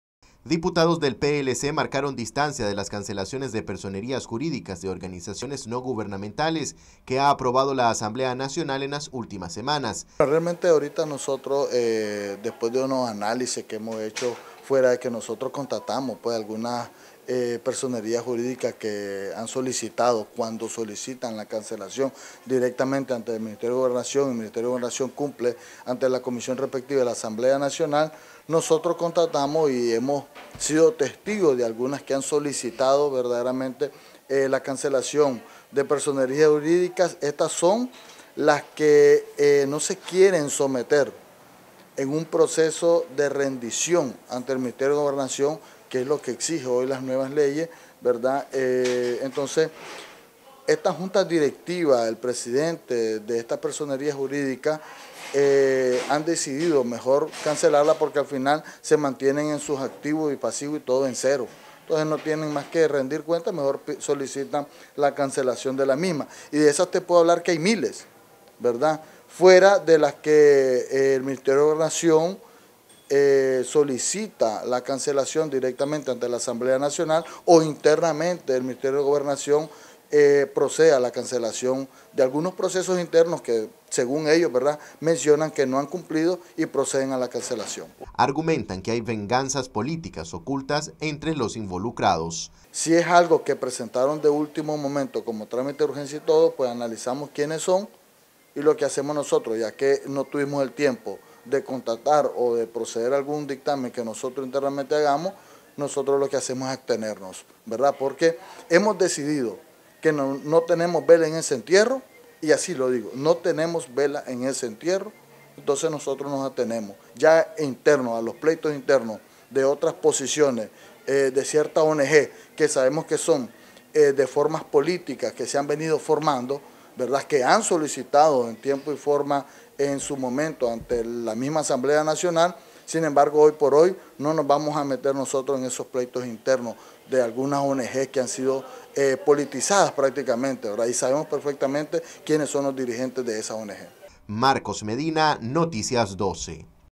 PLC se “desvincula” de cancelación de personerías jurídicas para ONG´S
Noticias12Nicaragua PLC se “desvincula” de cancelación de personerías jurídicas para ONG´s.